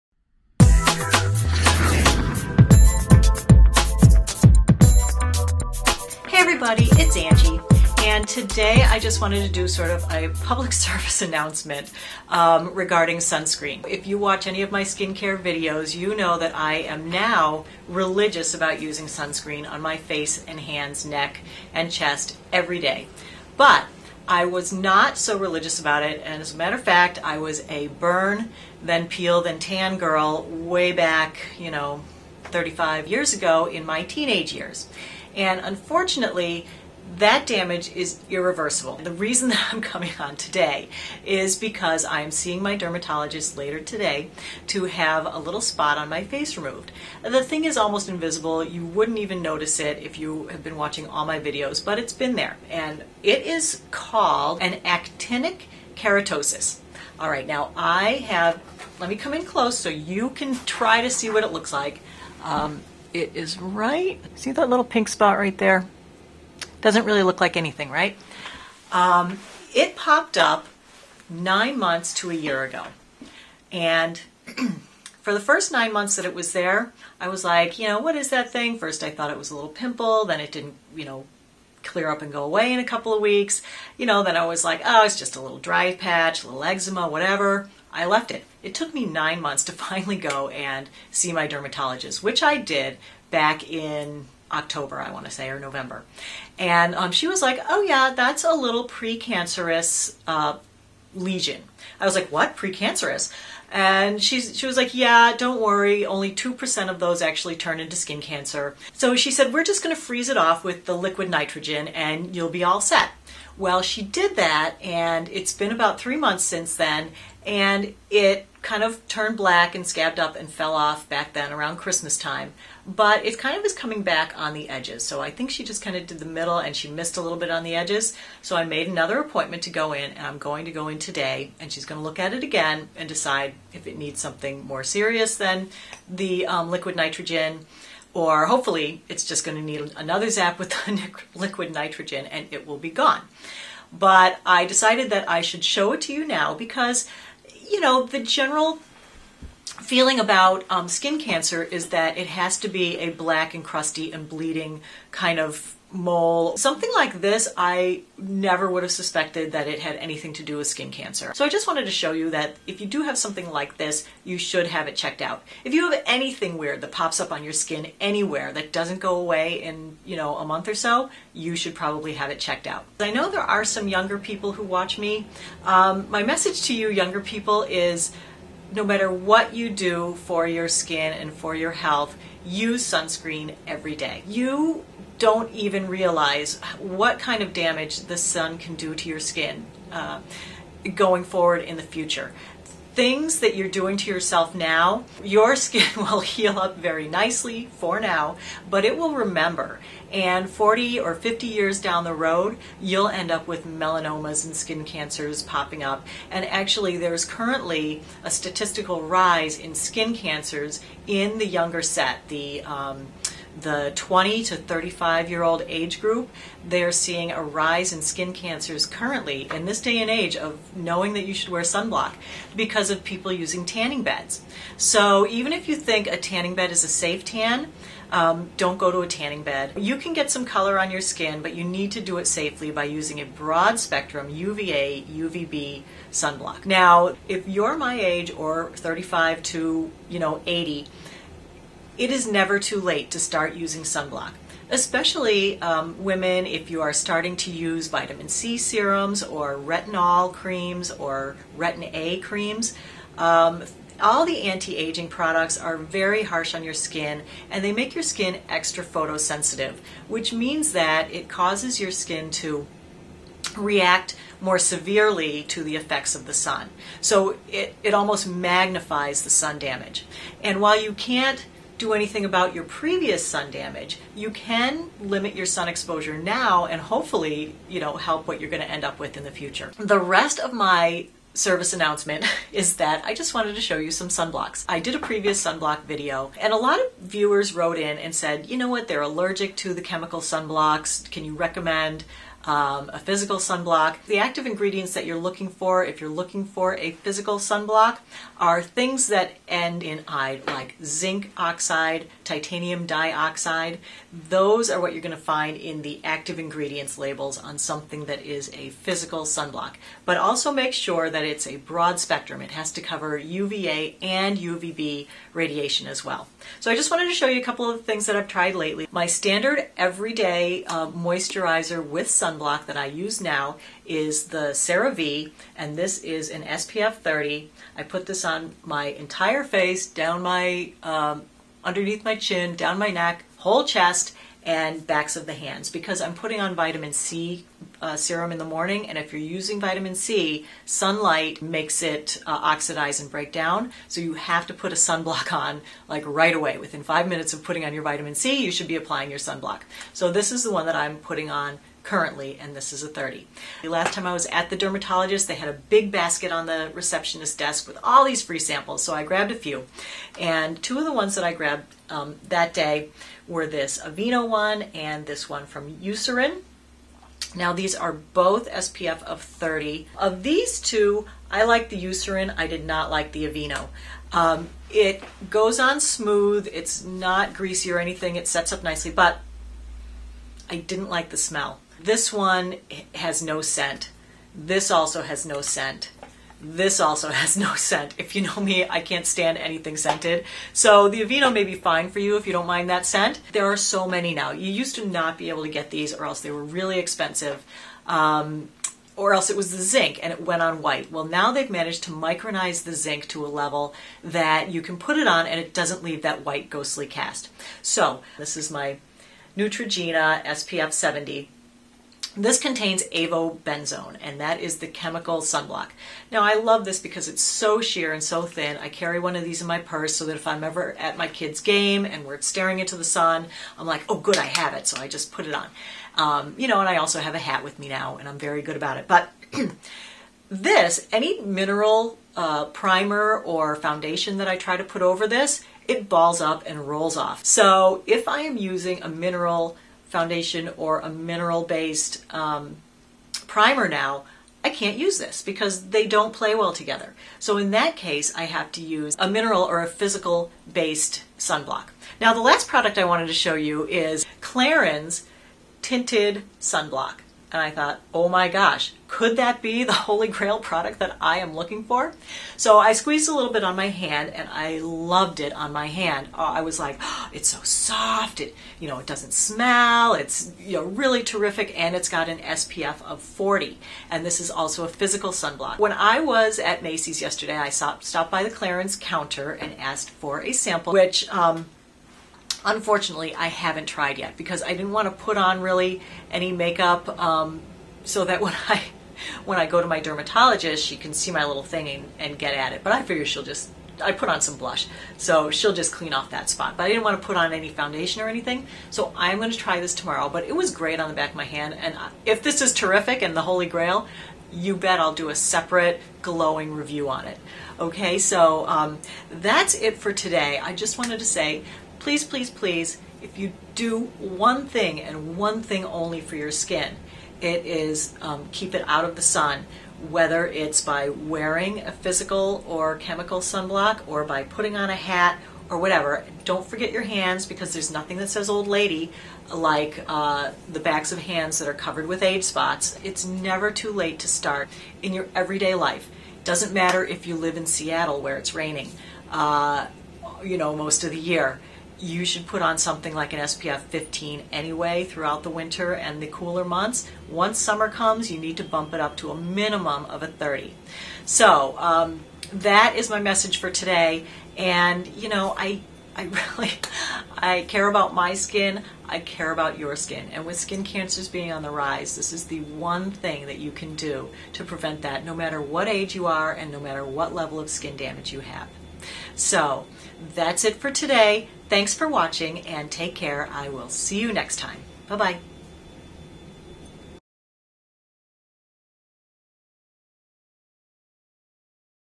Skin Cancer, My Mystery Spot & Why You Should Use Sunscreen
Style, Beauty, Anti-Aging, & Health for Women in Their Hot-Flash Years!

Don't worry, my dermatologist zapped it again with the liquid nitrogen and I should be good to go. I haven't really put my un-protected face in the sun in 15 years, so this is definitely from damage done long ago. Definitely start using sunscreen every day.